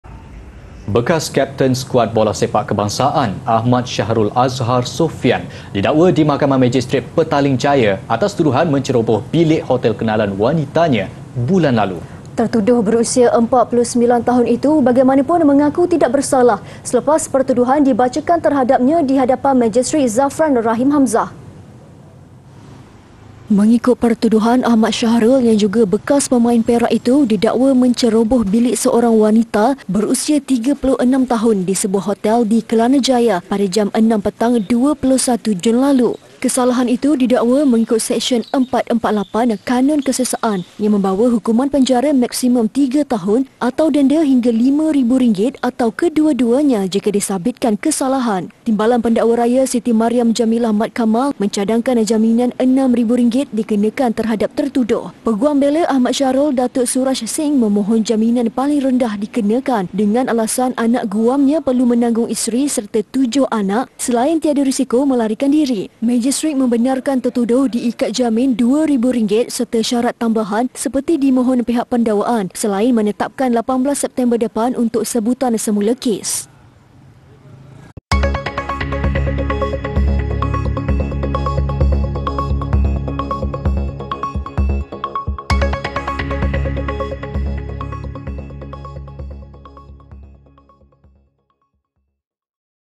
Bekas Kapten Bola Sepak Didakwa Ceroboh Bilik Hotel Kenalan Wanita
Bekas kapten skuad bola sepak kebangsaan Ahmad Shahrul Azhar Sofian didakwa di Mahkamah Majistret Petaling Jaya atas tuduhan menceroboh bilik hotel kenalan wanitanya bulan lalu.